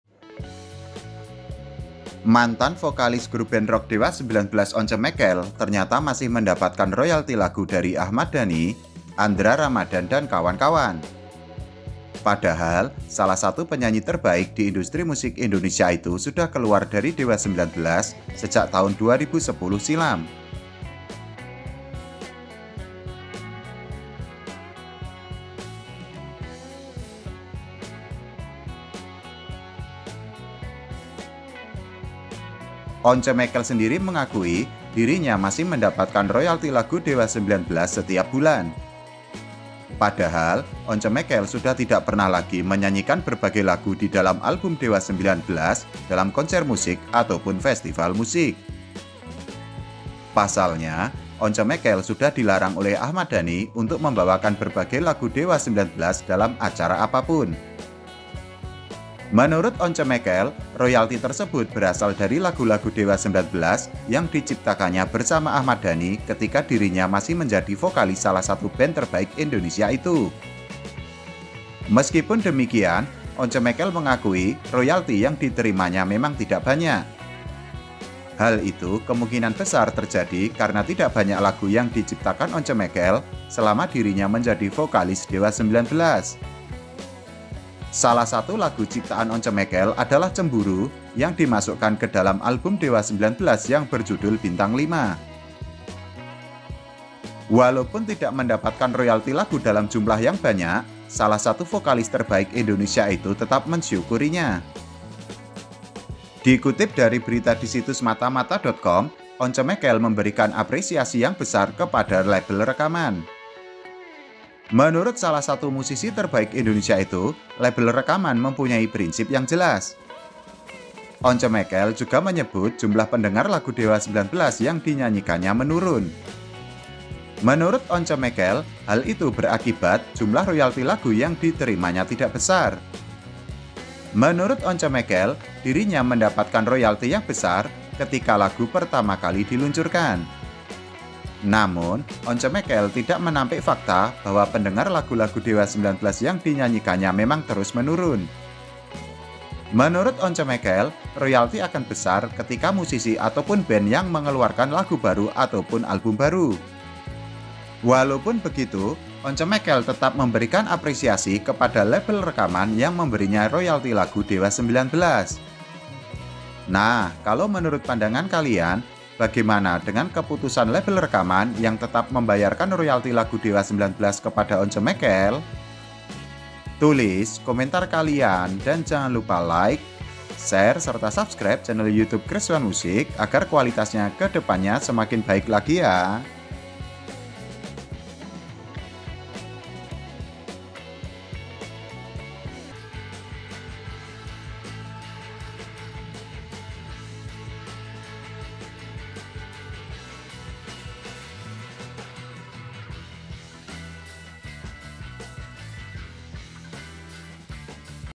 Dewa 19 ‼️ Once Mekel Terima Royalti Lagu Dari Ahmad Dhani, Tapi Kecil ‼️‼️‼️
Mantan vokalis grup band rock Dewa 19 Once Mekel ternyata masih mendapatkan royalti lagu dari Ahmad Dhani, Andra Ramadhan, dan kawan-kawan. Padahal, salah satu penyanyi terbaik di industri musik Indonesia itu sudah keluar dari Dewa 19 sejak tahun 2010 silam.
Once Mekel mengakui dirinya masih mendapatkan royalti lagu Dewa 19 setiap bulan. Padahal, Once Mekel sudah tidak pernah lagi menyanyikan berbagai lagu di dalam album Dewa 19 dalam konser musik ataupun festival musik. Pasalnya, Once Mekel sudah dilarang Ahmad Dhani membawakan berbagai lagu Dewa 19 dalam acara apa pun. Menurut Once Mekel, royalti tersebut berasal dari lagu-lagu Dewa 19 yang diciptakannya bersama Ahmad Dhani ketika dirinya masih menjadi vokalis salah satu band terbaik Indonesia itu.